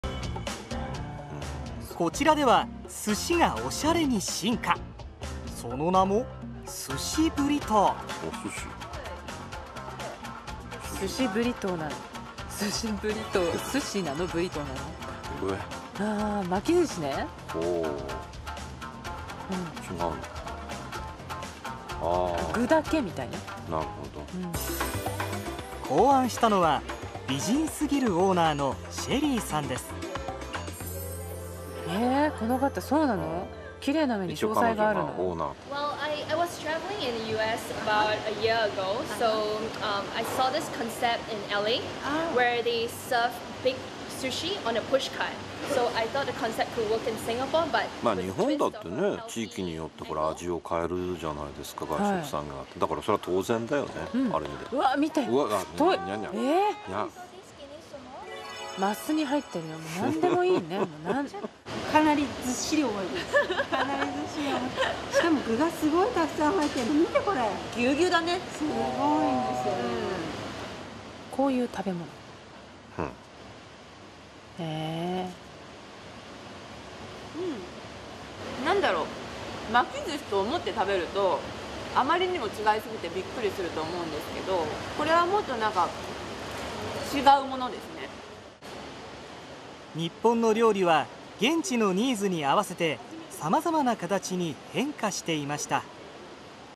Japanese TV Asian Times X Sushi Burrito Interview
Cute Ninja Girls get a taste of fusion Japanese Fast Food Sushi Burrito for the first time.. Watch their yummylicious bites!